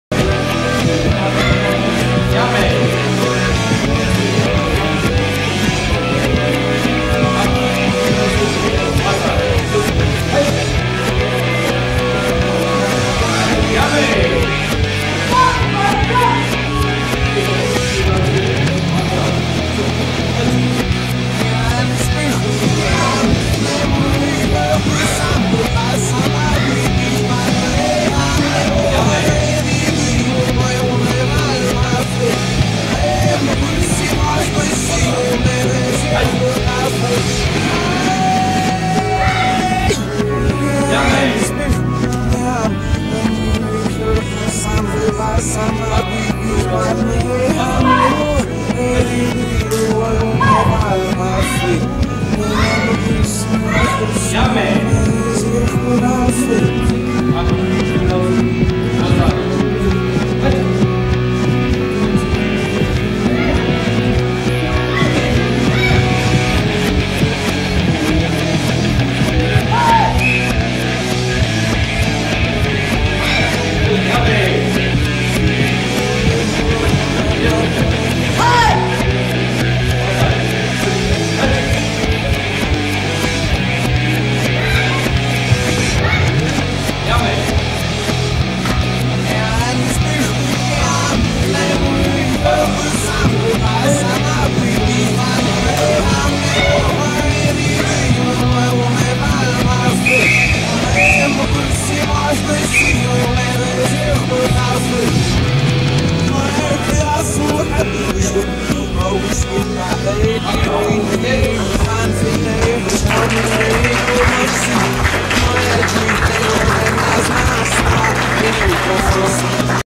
gkr wgtn dc 2019 06 23 r2 10 13yrs male 6th+ kata 04c seiunchin saifa
GKR Karate
Wellington, New Zealand
Dojo Competition
Postgate School
Staithes Dr N, Whitby
Porirua 5024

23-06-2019

Ring2: 10 - 13yrs Male 6th kyu+ Kata & Kumite